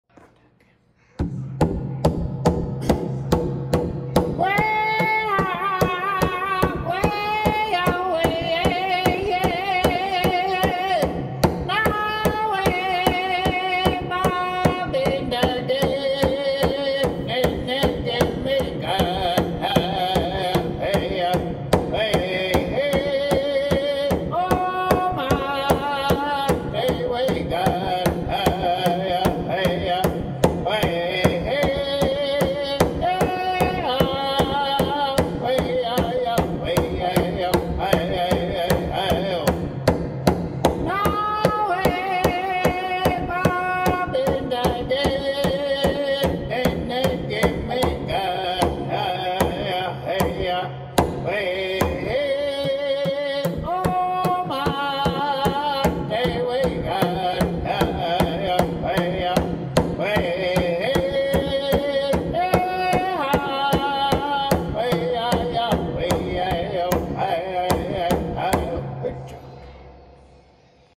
men's challenging song/2021
there he is... coming in, the man sneaking up on the drum... composed for any challenges from men dancers, if you know... you know.